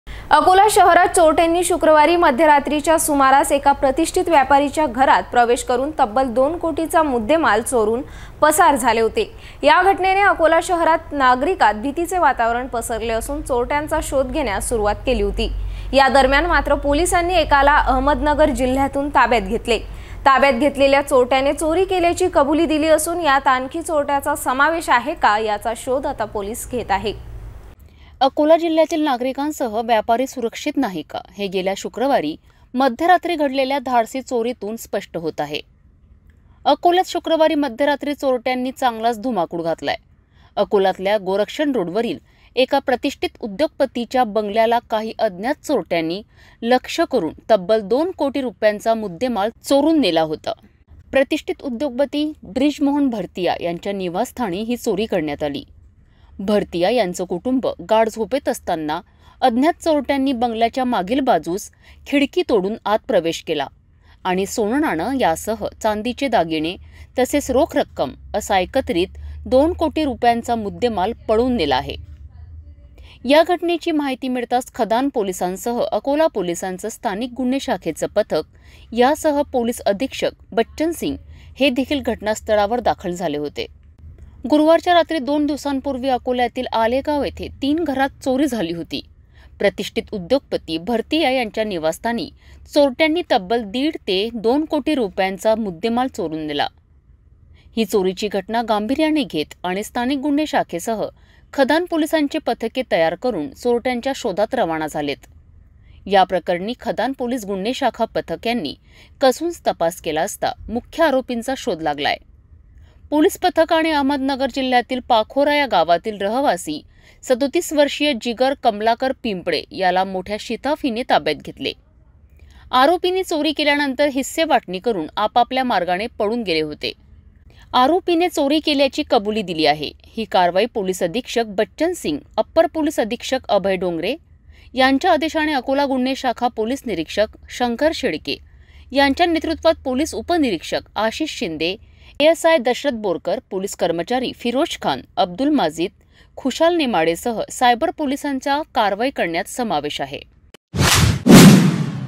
अकोल्यातून प्रतिष्ठित उद्योगपतीच्या बंगल्यावर चोरी प्रकरणात 1 ताब्यात
2 कोटि रुपयांची दिली कबुली ...